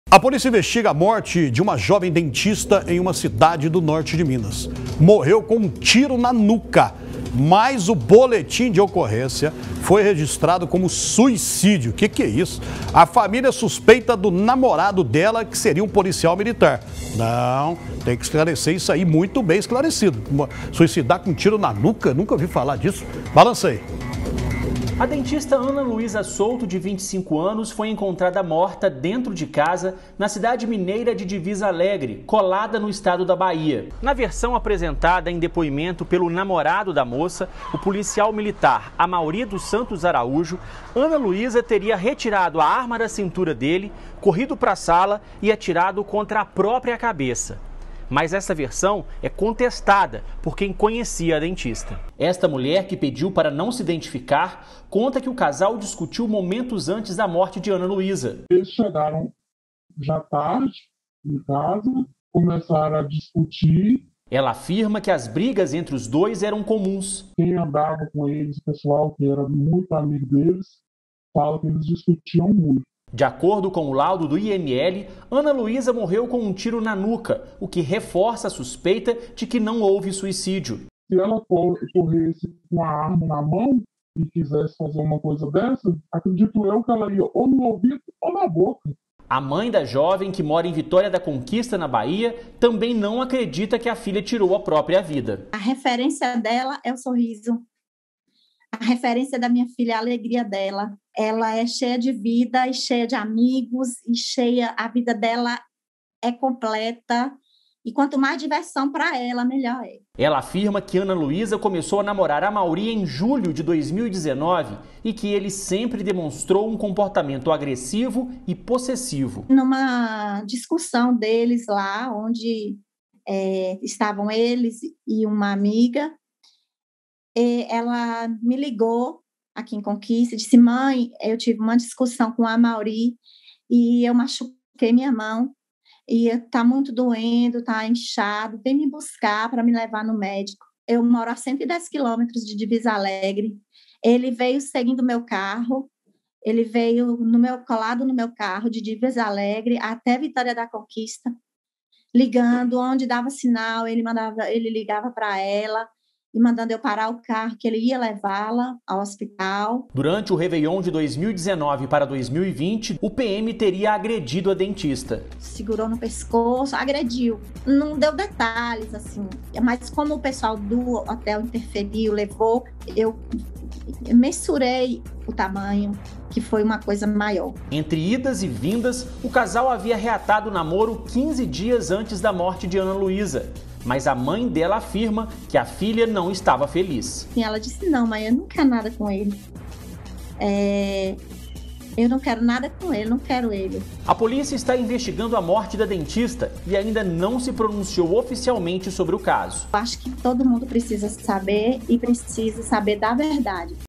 Jovem morre com tiro na nuca e família suspeita de ex-namorado
A Polícia Civil investiga a morte de uma jovem dentista em Divisa Alegre, a 700 km de Belo Horizonte. Ela morreu com um tiro na nuca, mas o boletim de ocorrência foi registrado como suicídio. A família suspeita que o namorado dela, que é militar, tenha cometido o crime.